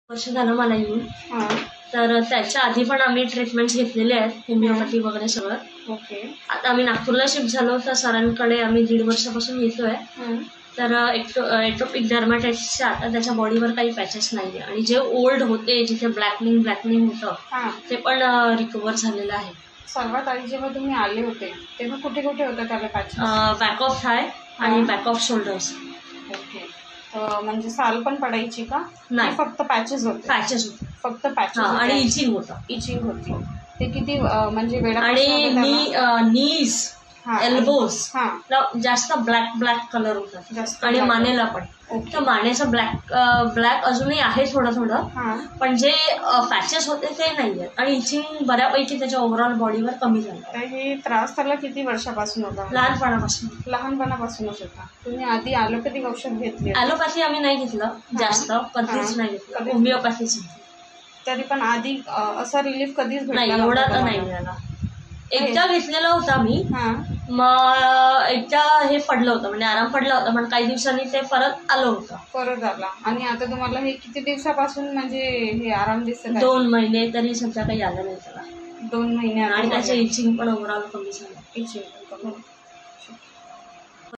Homoeopathy treatment for ATOPIC DERMATITIS | डॉ. अब्देकायम चिमथानावाला
the testimonial of the pt shows the effectiveness of Homoeopathic treatment